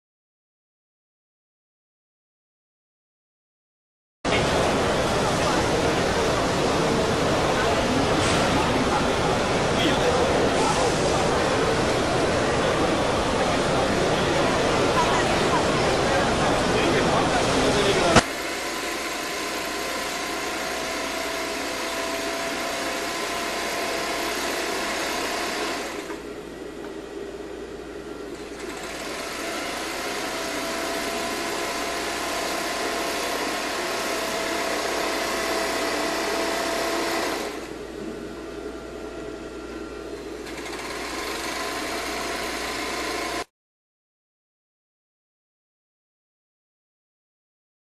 Galoneira Kansai Special WX8803EMK
Galoneira de base plana tipo BT, convencional. Com refilador de tecido do lado direito, trançador superior, tensor frontal de alimentação de elástico e renda com regulagem de franzidos mais precisa através do sistema ZEROMAX. Indicada para aplicar elástico aparente e renda em lingerie em geral.